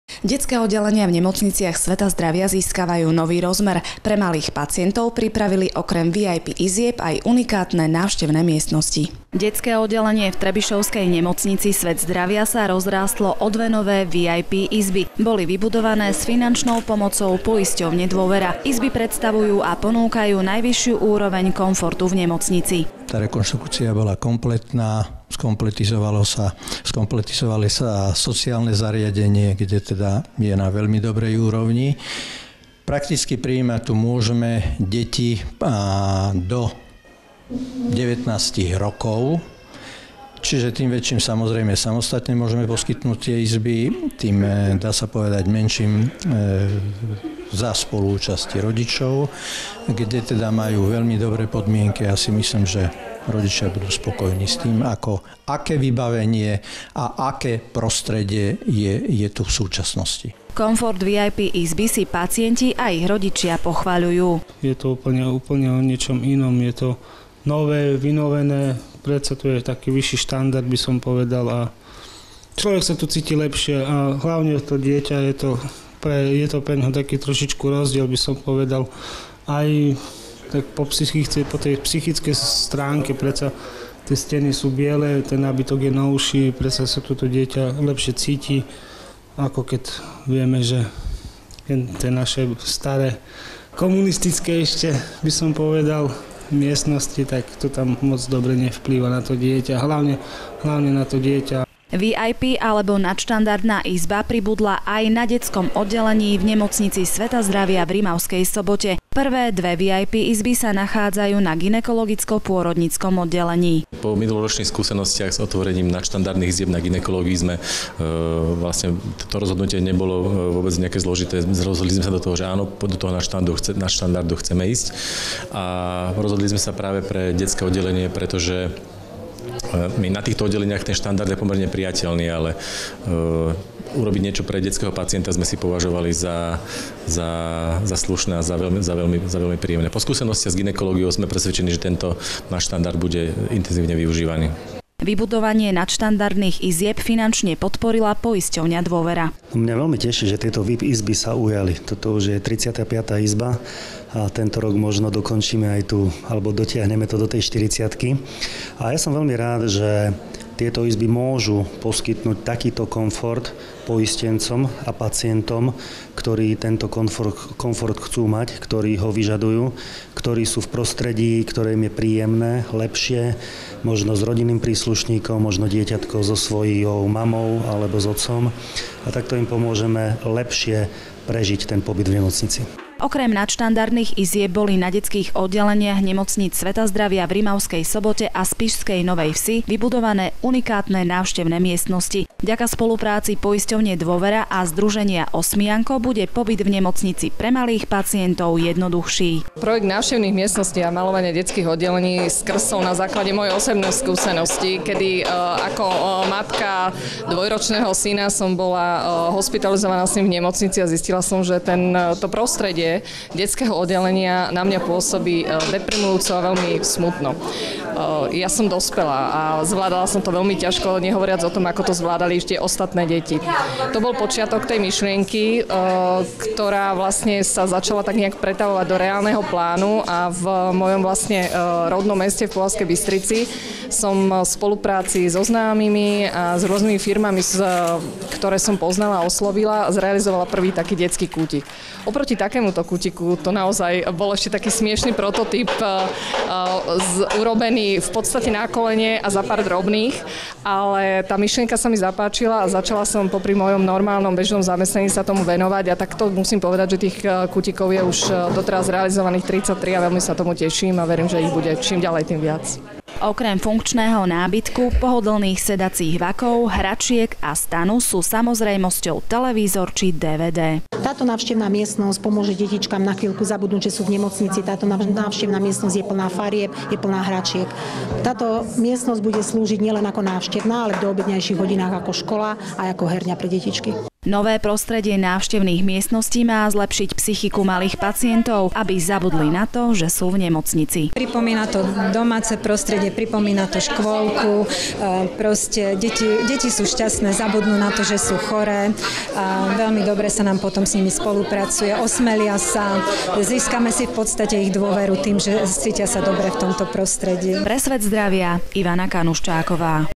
Nové návštevné miestnosti a VIP izby Dôvera v nemocniciach
Vďaka spolupráci zdravotnej poisťovne Dôvera a združenia Osmijanko bude pobyt v nemocniciach Svet zdravia pre malých pacientov jednoduchší. Na detských oddeleniach postupne vznikajú nové unikátne návštevné miestnosti, kde sa môžu hrať aj učiť. Nové liečivé prostredie má výrazne zlepšiť detskú psychiku počas hospitalizácie. Súcasne sa budujú aj nadštandardné VIP izby. Viac informácií nájdete na